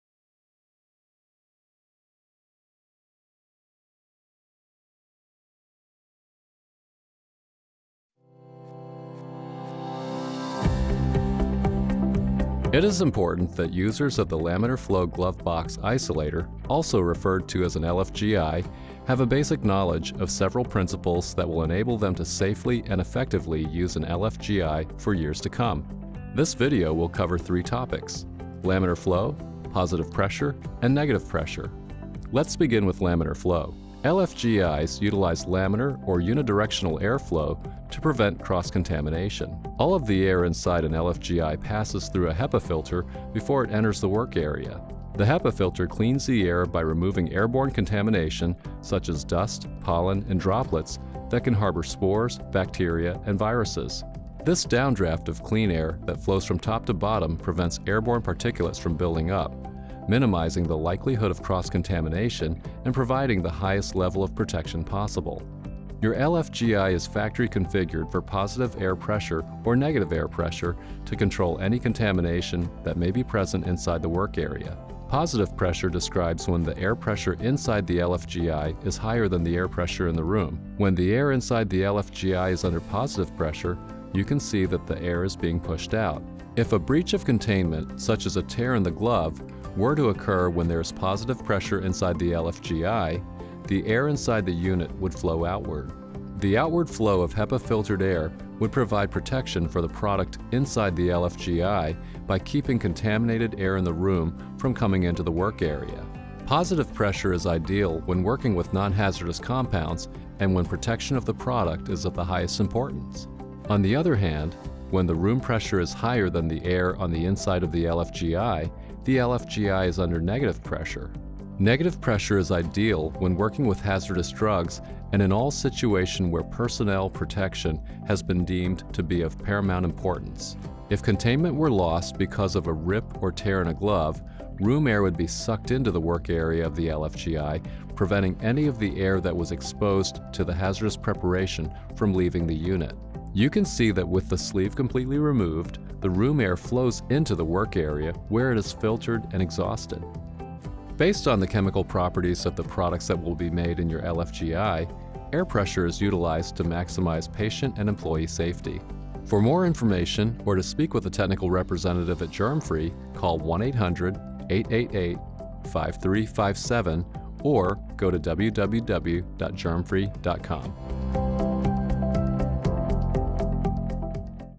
Germfree - LFGI Principles of Operation
An overview of the basic operating principles of Germfree's LFGI Compounding Aseptic Isolator / Compounding Aseptic Containment Isolator.  Topics include: Laminar Flow, Positive Pressure and Negative Pressure.